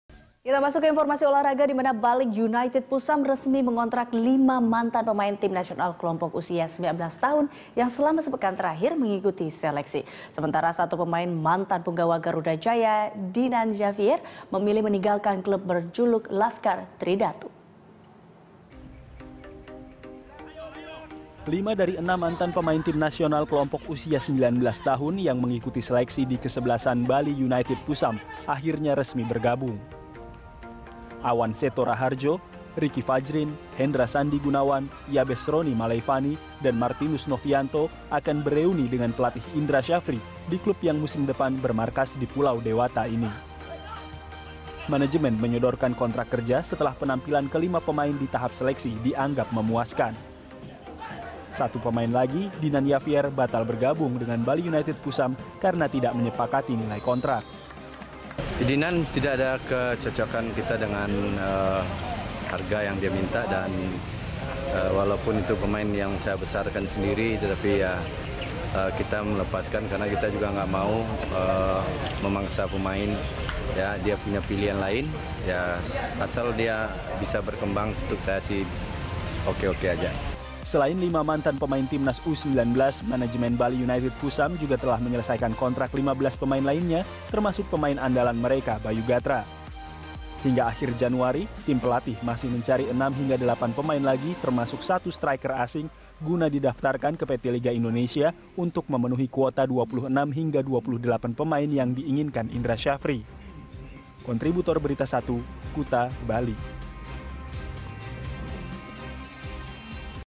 Bali United Pusam Mengontrak Lima Mantan Pemain Timnas U-19
Bali United Pusam resmi mengontrak lima mantan pemain Tim Nasional kelompok usia 19 tahun yang selama sepekan terakhir mengikuti seleksi. Sementara itu, satu pemain mantan punggawa Garuda Jaya Dinan Javier memilih hengkang dari klub berjuluk Laskar Tri Datu.

Facebook.com/BeritaSatuTV
Youtube.com/BeritaSatu